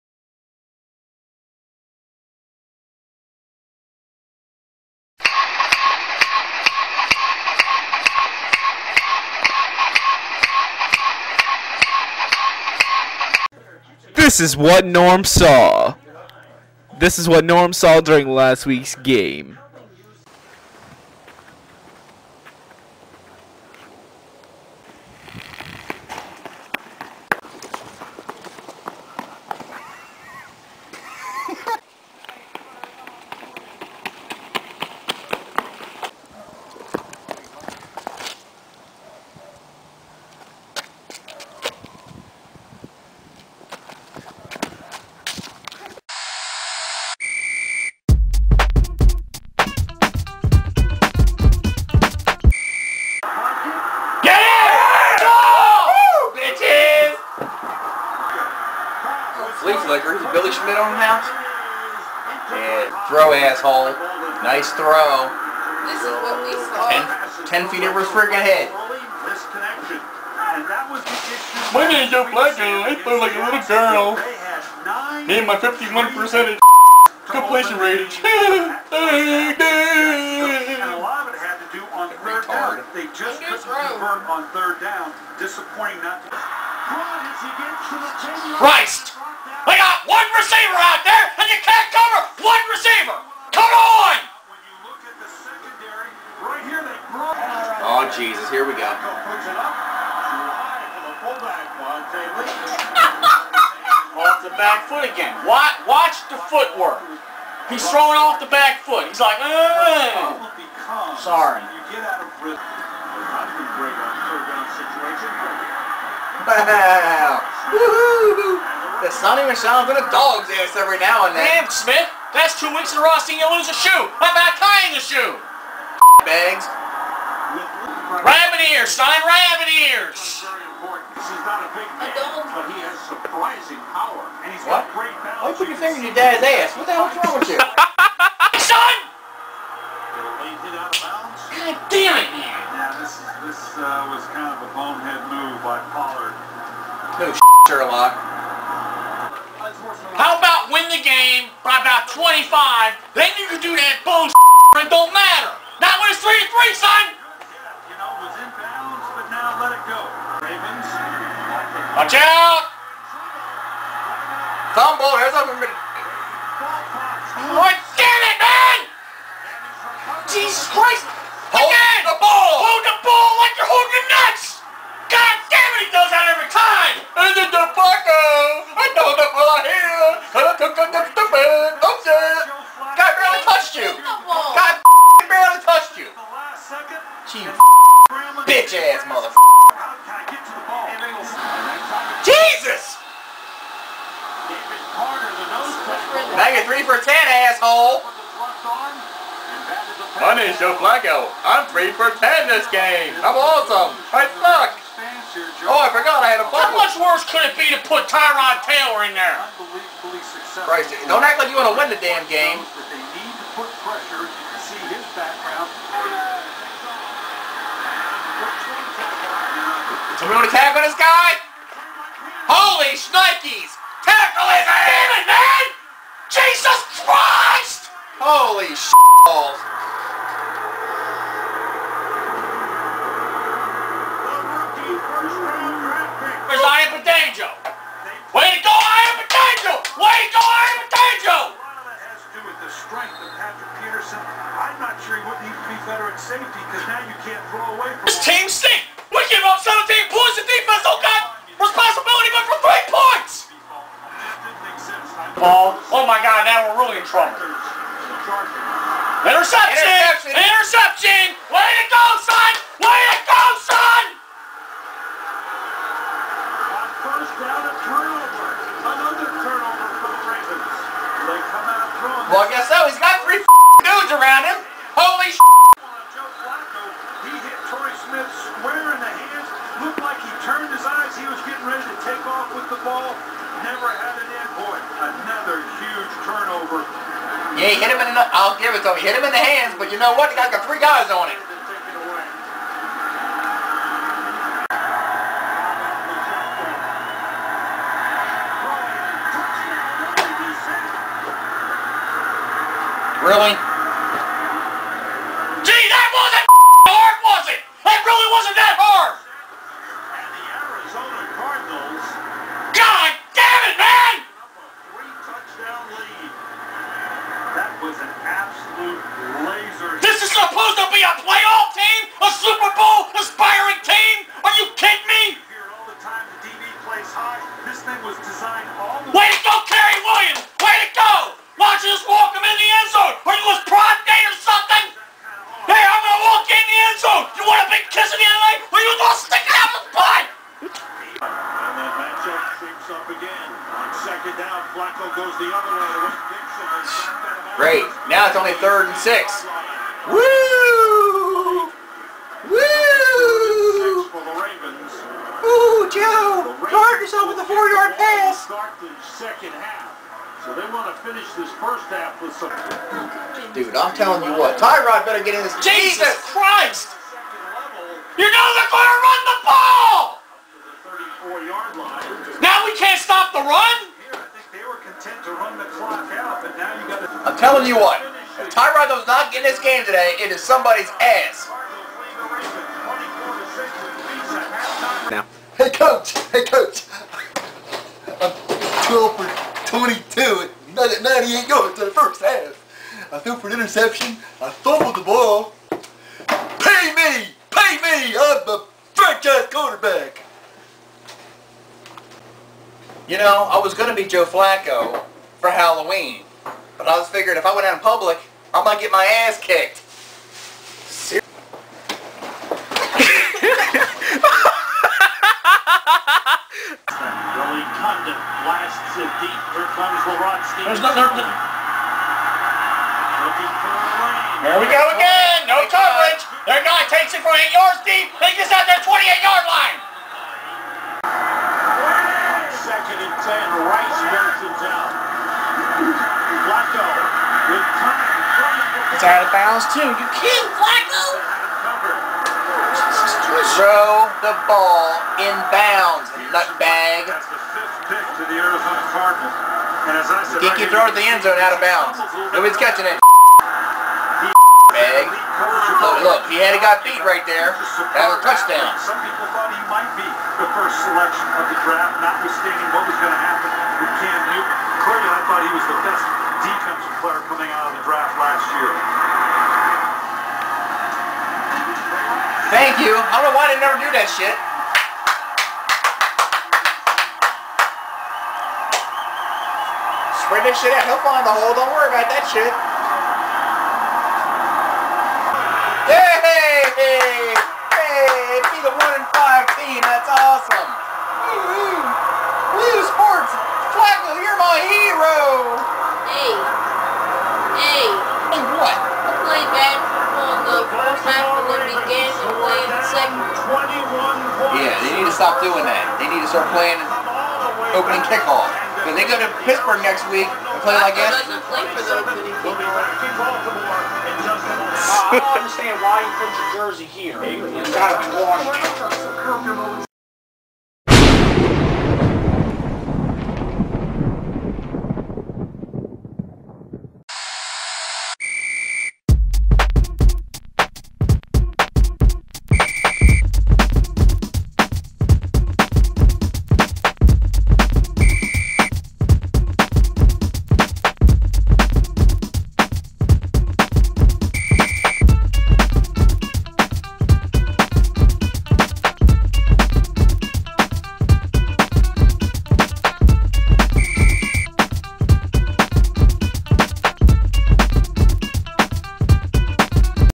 Scotty D@mn G Has PURPLE RAGE Episode 8: Week 8 vs. Arizona Cardinals
The new episode of Scotty D@mn G Has Purple Rage. We took on the Cardinals this week, and this is what happened. Also, Norm showed up for the WHOLE video this week, so this should be interesting.